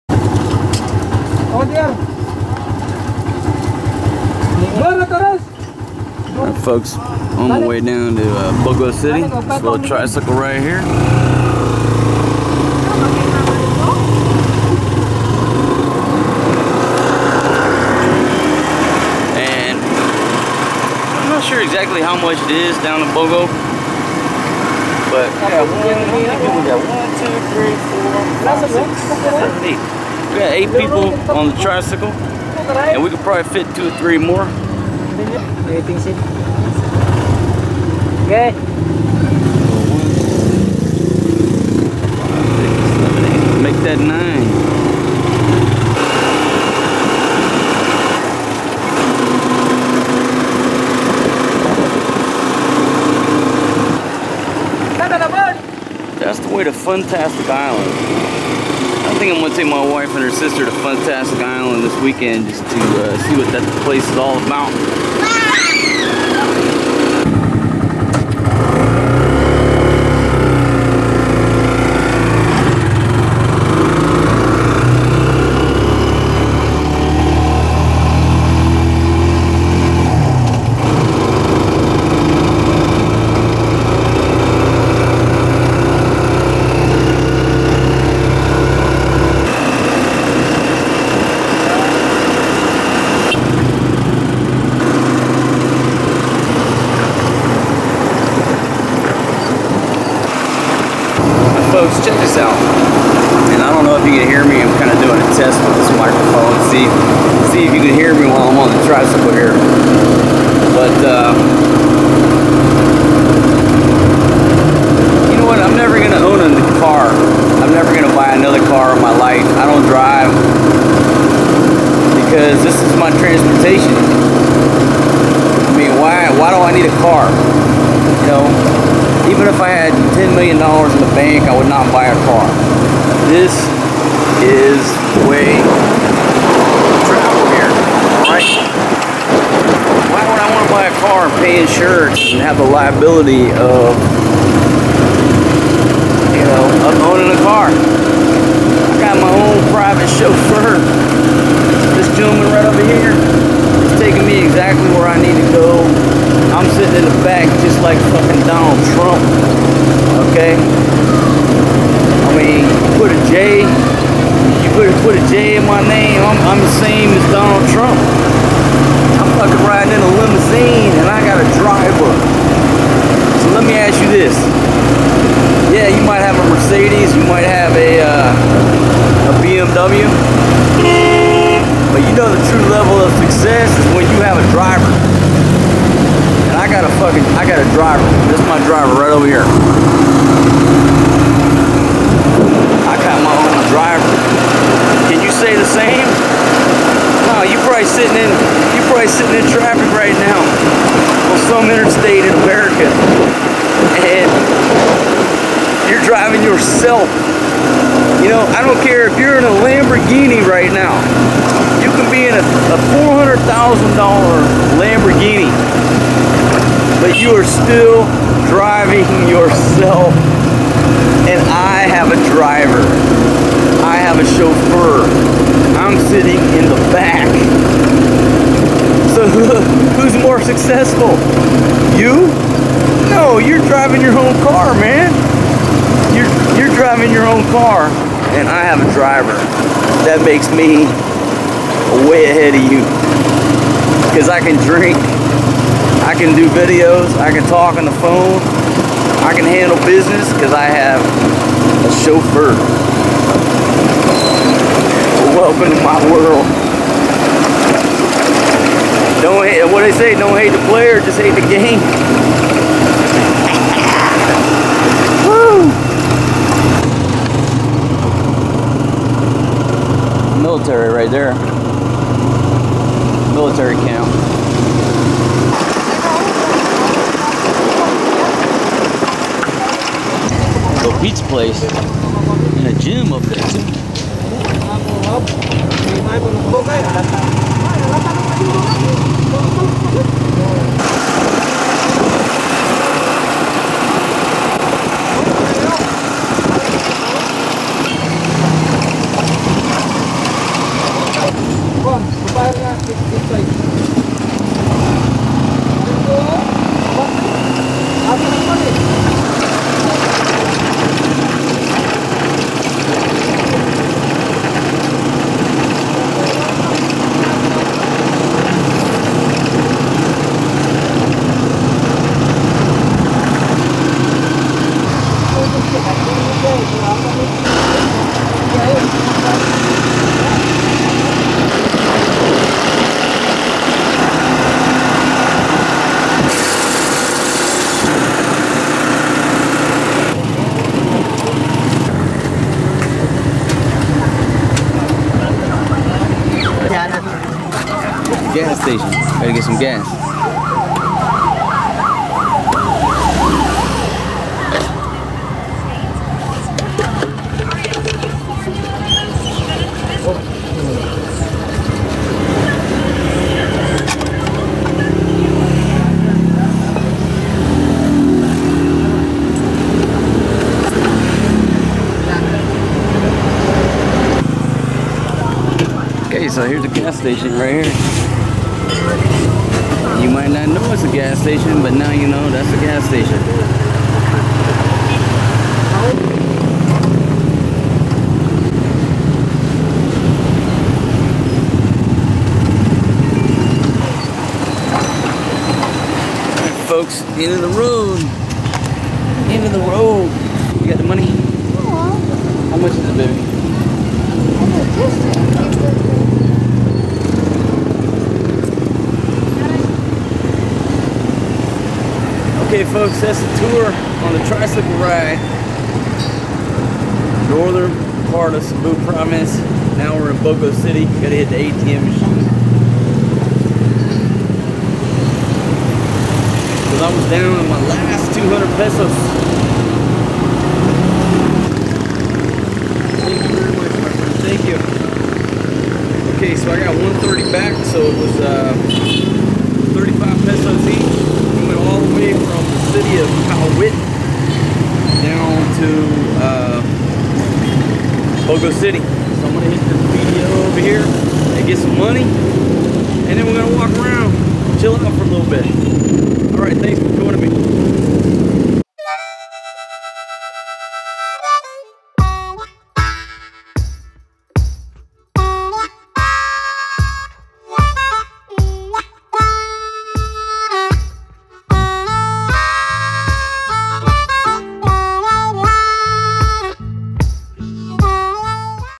Riding in the Back of My Limousine in the Philippines. Like a Boss.
Had to ride in my limo down to Bogo City to hit the ATM machine. Take a ride with me through the countryside, here in the Philippines.